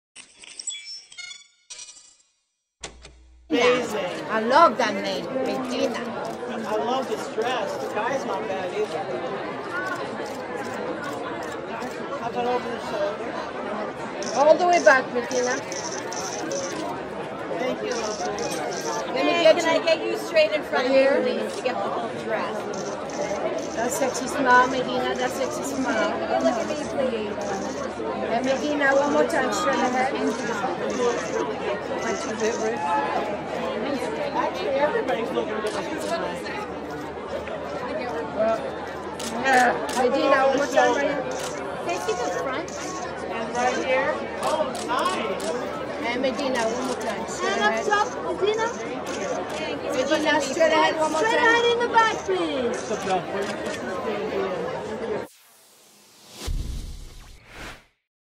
Medina Senghore "The Walking Dead" Series Finale Event in Los Angeles
Broll footage: Medina Senghore on the black carpet at "The Walking Dead" series finale event held at The Orpheum Theatre in Los Angeles, California USA on November 20, 2022.